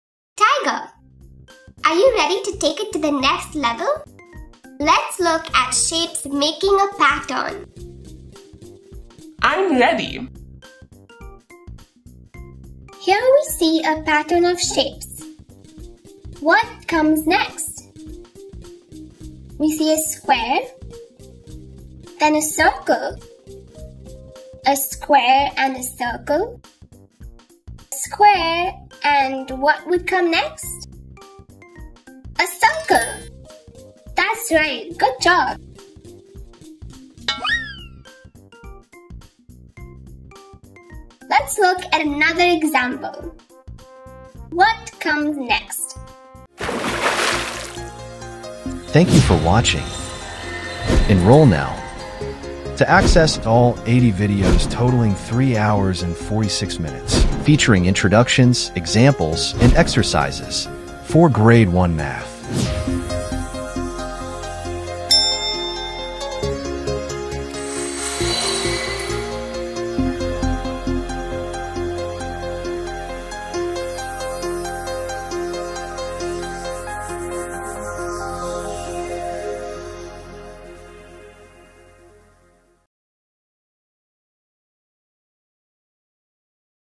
G1 - Module 6 - Exercise 4 - Complete the Pattern | Appu Series | Grade 1 Math Learning
Explore sequences and trends as you complete the pattern, enhancing your problem-solving abilities. Join the Appu and Tiger in this exercise as they help you up your skill base!